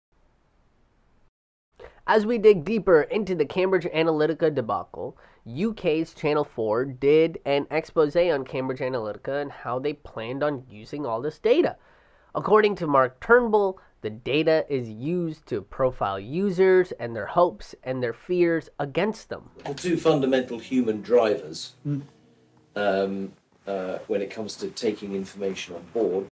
Why We Should've Killed Buzzfeed Quizzes
Buzzfeed Quizzes could have led to the issues with Cambridge Analytica and the hijacking of our psyches!